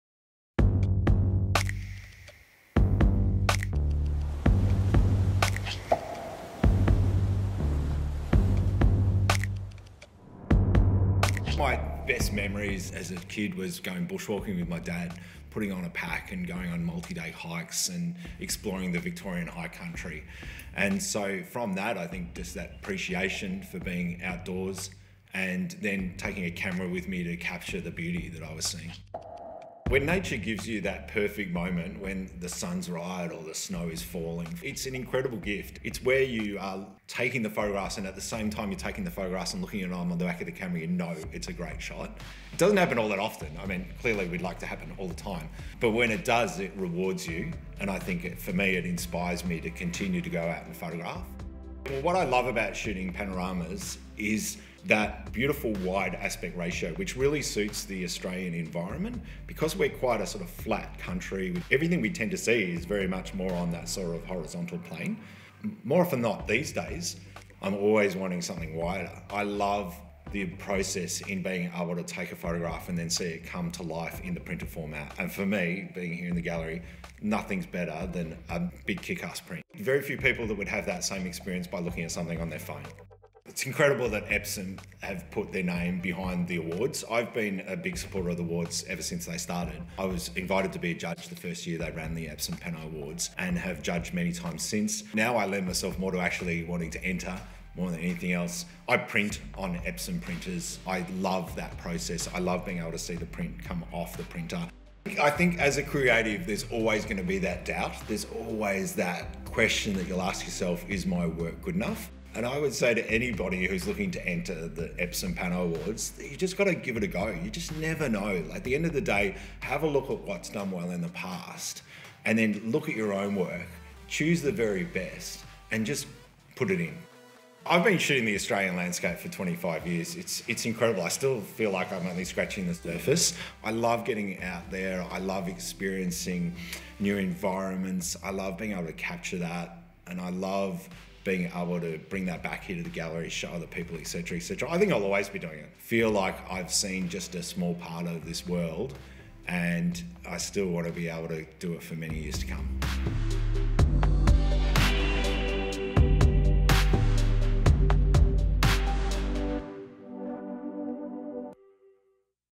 Tom Putt | The Epson International Pano Awards Series
The Epson International Pano Awards was founded in 2009 and has since continued to grow to what is now the world's largest competition devoted to the craft and art of panoramic photography!
Thousands of entries are received every year, competing for a variety of prizes and the opportunity to have their panoramic work showcased worldwide!
We recently spoke with award-winning judges, prize winners and creator of the awards David Evans, who shared their captivating stories about why they love this art form and competition so much.